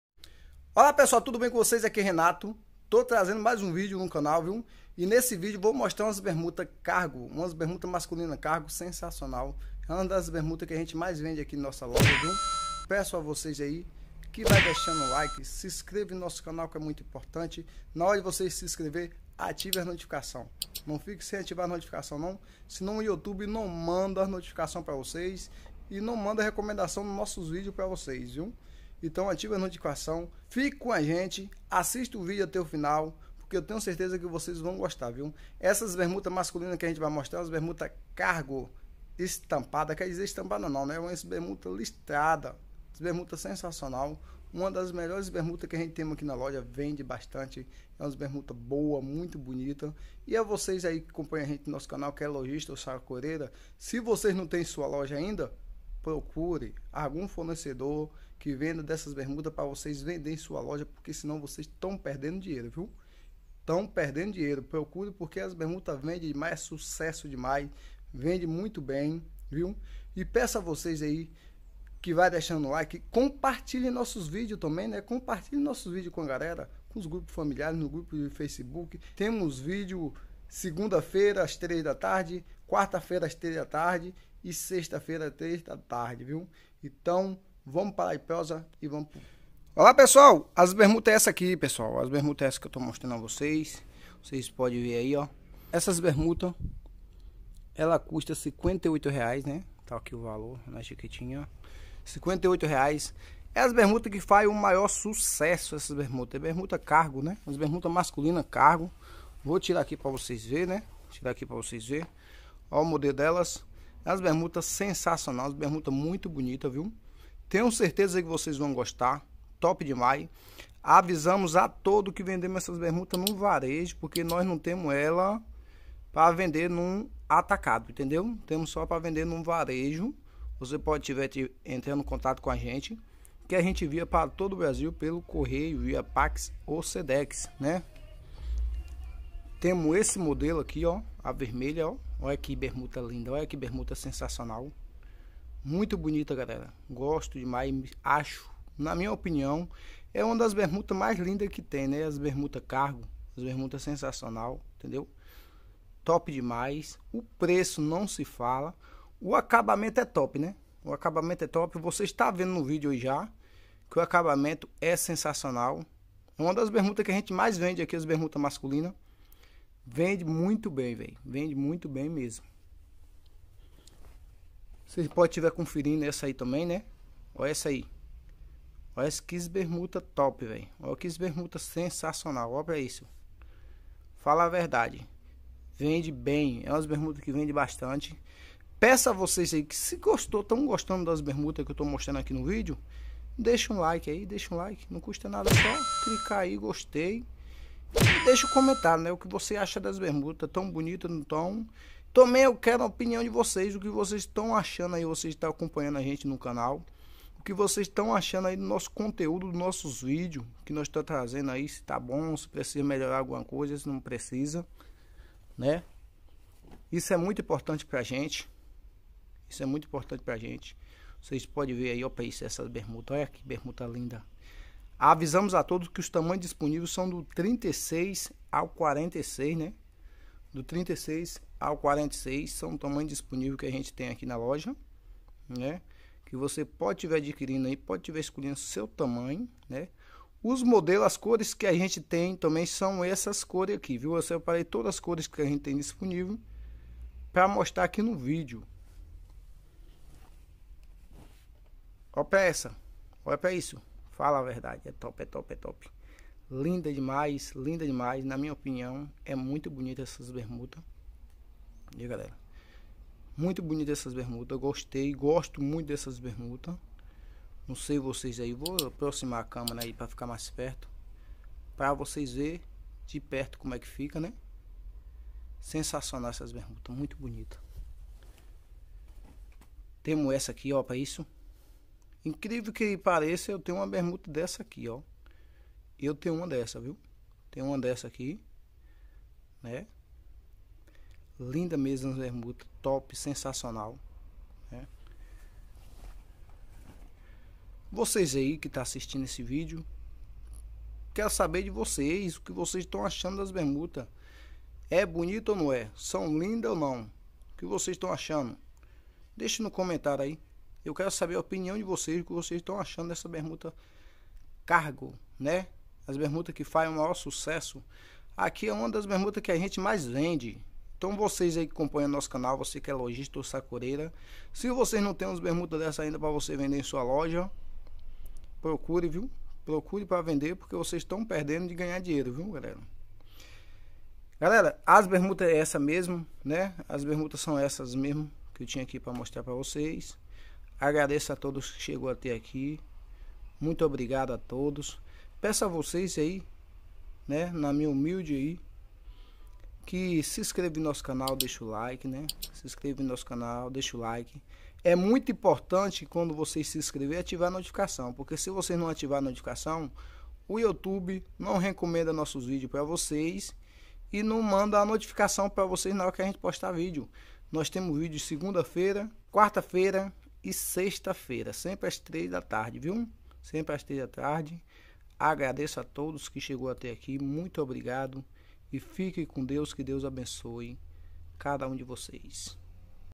Revendedor de bermuda cargo masculino/bermuda masculina cargo direto de fábrica.
Pessoal nesse vídeo você vai ver lindas bermuda masculina cargo sensacional uma mais linda que a outra viu eu garanto a vocês que vocês vão gostar das Bermudas masculina cargo porque é sucesso total.

 Essas bermudas é sucesso de vendas então  você que está assistindo esse vídeo se você não tem na sua loja ainda você tá perdendo de ganhar dinheiro viu então assista o vídeo até o final e fique com a gente que vocês vão gostar.

compartilhe nos grupos Facebook WhatsApp no grupo familiar

Siga a gente nas rede social